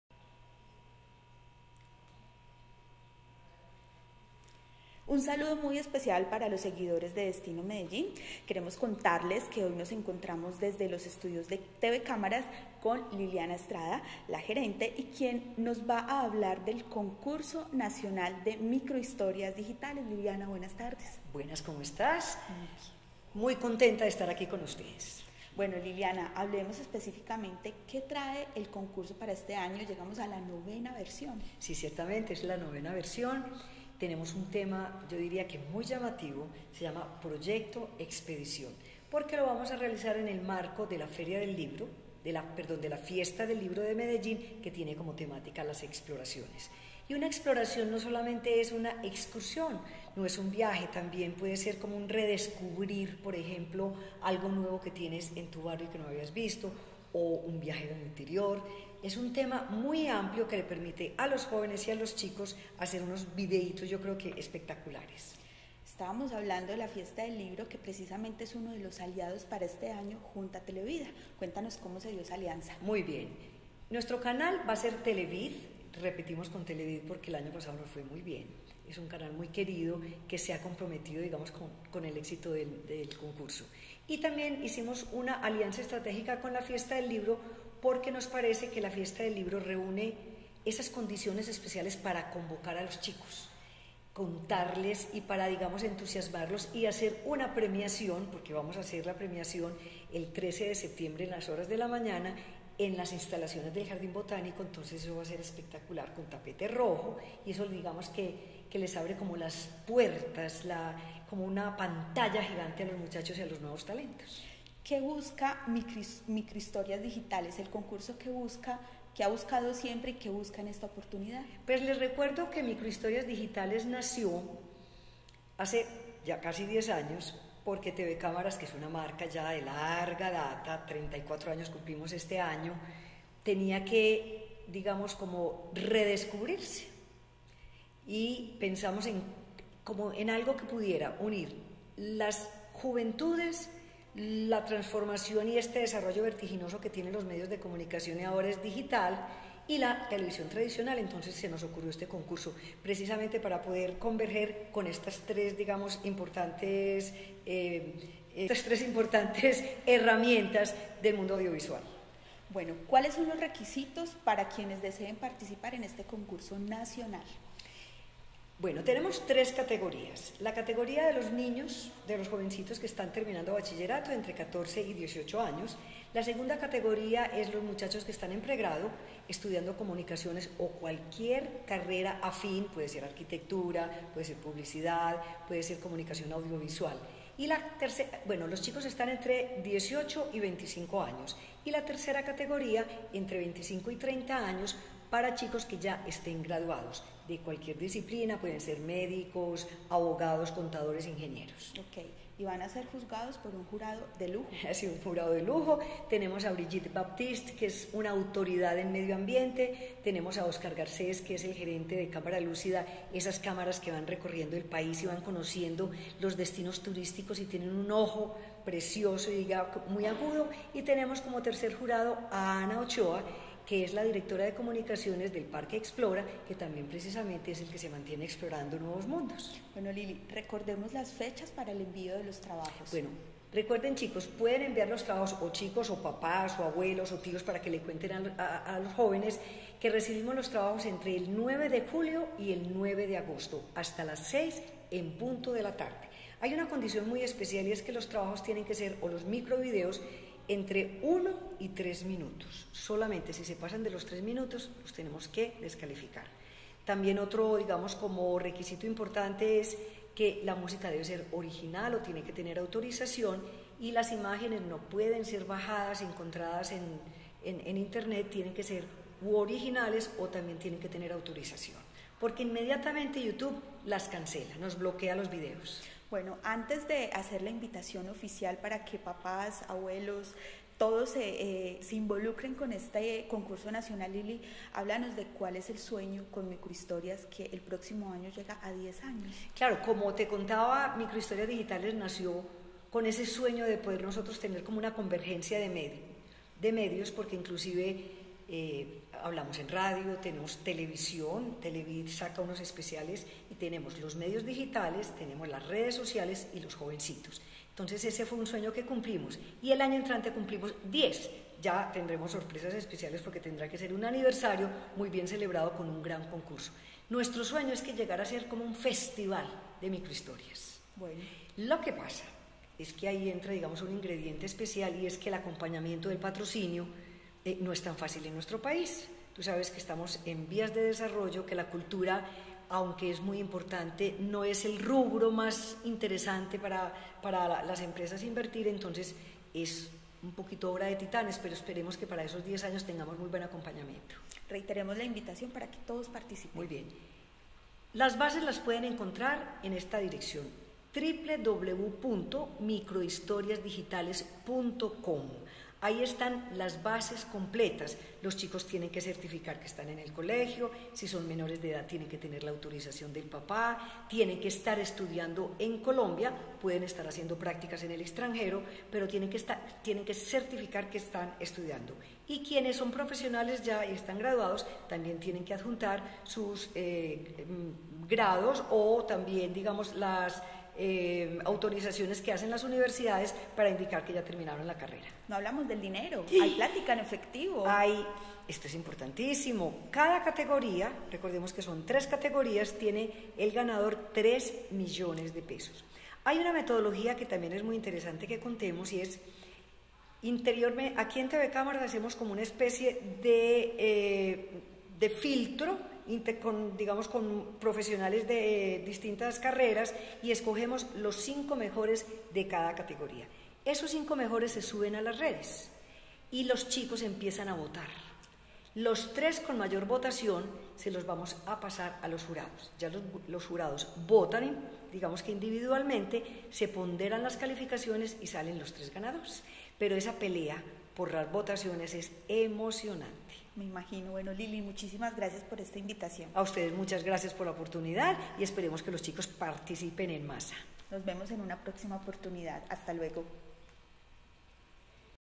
Microhistorias Digitales 2019
En su novena versión MICROHISTORIAS DIGITALES, invita a los participantes a viajar, a explorar su entorno geográfico o a realizar una introspección en su interior.

Quiénes pueden participar
* Estudiantes entre los 14 y los 28 años, y realizadores cuya edad no supere los 30 años, de nacionalidad colombiana, pero pueden estar residiendo en cualquier país.

* Los jóvenes menores de edad deberán adjuntar un certificado, autenticado en notaría, en el que se nos informe que sus padres autorizan la participación en el concurso.

* Para participar en la Categoría Estudiantes de Secundaria deben estar matriculados en cualquier institución educativa y tener entre 14 y 18 años

* Para participar en la Categoría Estudiantes de Educación Superior : Adelantar carreras universitarias, técnicas o tecnológicas de Comunicación, Diseño, Artes Plásticas, Cine, Publicidad, Video, Fotografía y Televisión, y tener entre 16 y 28 años.

* Para participar en la Categoría Realizadores: Los profesionales deberán contar con título universitario en cualquier disciplina y no superar los 30 años de edad.

* Solo se admitirá un trabajo por estudiante o por grupo, para las tres categorías.

* No pueden participar personas vinculadas a TV CÁMARAS ni a TELEVID, ni sus familiares con algún grado de consanguinidad.

* Los trabajos de autoría colectiva, deberán estar acompañados de una carta, en la cual se señale en forma precisa el trabajo realizado por cada uno de los participantes.

* Las microhistorias deben ser inéditas, no haber participado en otros concursos y no exceder el límite de tiempo: tres (3) minutos. Pueden ser trabajos que hayan sido realizados para una asignatura académica.

* Si la entrega del video es física, la propuesta se recibirá en DVD o USB, y si es digital, el video se debe entregar en formato Mov o MP4 y debe enviarse al correo

* El jurado premiará la creatividad, la originalidad de la idea, el lenguaje narrativo y audiovisual, la calidad técnica y el impacto social de la propuesta.

* Las propuestas inscritas para concursar no se devolverán, y si forman parte de la preselección inicial estarán publicadas en los medios web, pantallas y otras plataformas de exhibición de TV Cámaras y Televid, sin límite de tiempo y hasta que estas empresas lo consideren pertinente

* Por el hecho de enviar los trabajos al Concurso, los participantes aceptan las reglas del mismo. Esta autorización deberá expresarse por escrito en el formulario de inscripción.

* El incumplimiento de cualquiera de estas condiciones, descalifica el trabajo presentado.

* La música y las imágenes que se usen para las microhistorias deberán ser originales, de lo contrario deberán adjuntar una certificación de cesión de derechos por parte de quien los ostenta.

INSCRIPCIÓN Y PRESENTACIÓN DE LOS TRABAJOS
Para inscribirse se deben seguir las instrucciones que aparecen o en la página principal de TV CÁMARAS, siguiendo el link Concurso.

*Las propuestas pueden presentarse en cualquier medio digital (Físico o enviándose por correo electrónico). Deben tener una duración mínima de un minuto y máxima de tres minutos.

* Para las propuestas que se entregan de forma física (DVD o USB ) , se debe adjuntar la confirmación impresa que certifique la inscripción por la WEB.

* Las propuestas deben venir acompañadas de una carta que incluya la identificación como autor (o autores en caso de trabajos colectivos); título de la historia y ubicación geográfica de su realización.

* Las microhistorias que deseen participar en el Concurso deberán diligenciar el formulario de inscripción que aparece en el micrositio del concurso ) y adjuntarlo al envío, bien sea físico o digital.

* Igualmente escribir una cuartilla (hoja tamaño carta a doble espacio – letra Arial 12) en la cual el autor o autores expresen la motivación y el sentido de la historia seleccionada.

* Anexar fotocopia del (o los) documento(s) de identidad del o los participantes.

* Indispensable: certificado original de estudio, de la institución educativa a la cual está (n) vinculado (s). Para trabajos en grupo se deben presentar certificado de cada uno de los integrantes. Para el caso de los profesionales adjuntar acta de grado

* Para entrega física, se deberá entregar un original y dos copias del video.